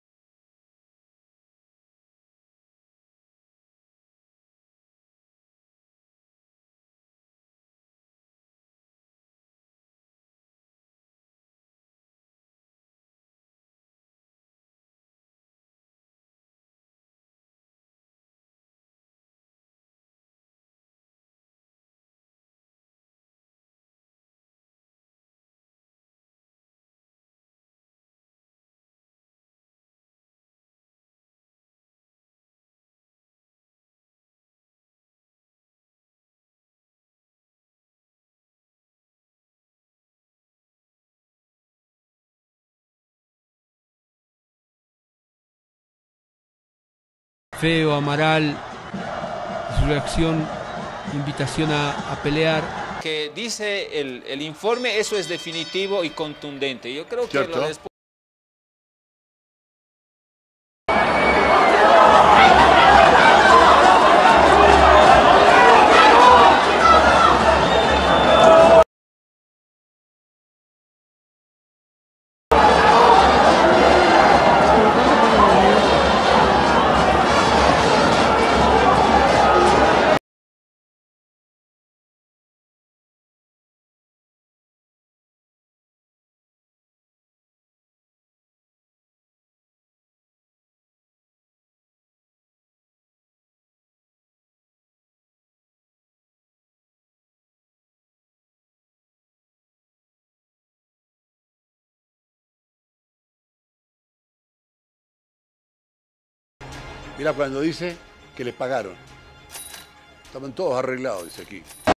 El caso Amaral complicado tras estas pruebas post Clásico Paceño
Bolivar presentó pruebas contra Rodrigo Amaral tras el Clasico Paceño